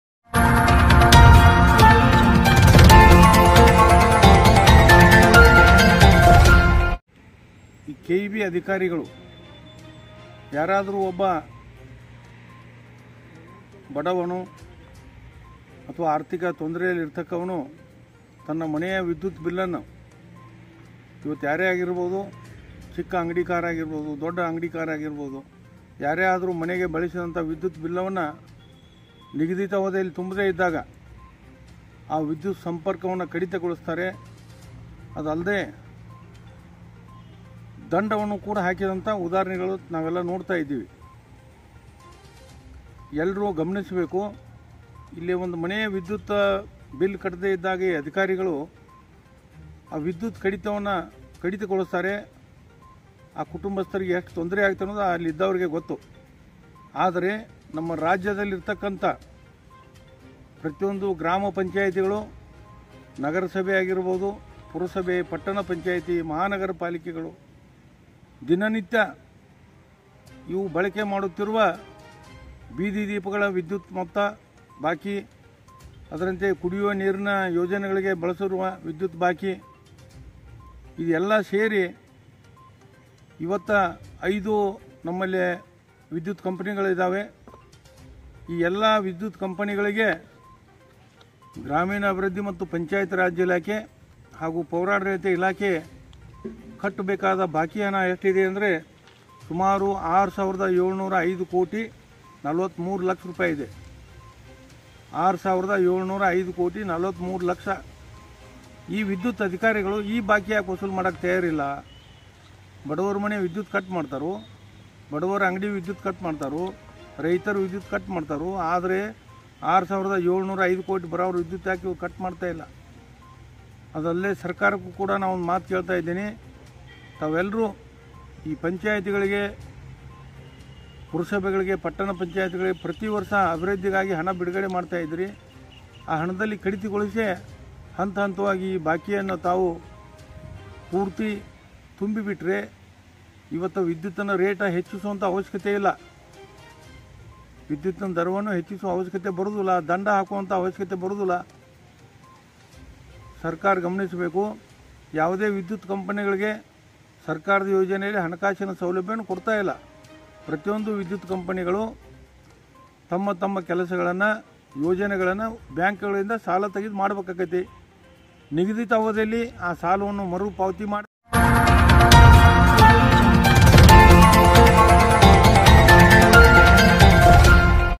ಹೆಸ್ಕಾಂ ಅಧಿಕಾರಿಗಳ ವಿರುದ್ದ ಸಾಮಾಜಿಕ ಹೋರಾಟಗಾರ ಭೀಮಪ್ಪ ಗಡಾದ ಗರಂ.
ಬೆಳಗಾವಿ